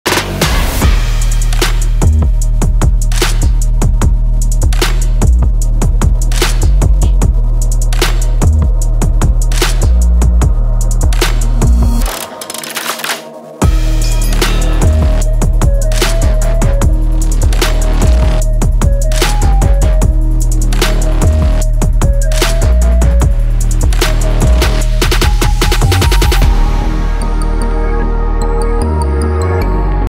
O'NEAL EX-SRS Helmet | Features
We‘re Introducing our new and innovative EX-SRS Helmet!
It is the 1st helmet on the market with integrated Airflaps and packed with features for your all-day riding adventure. Perfect for a wide range of conditions, temperatures and challenges with complete control for your individual needs. Experience it!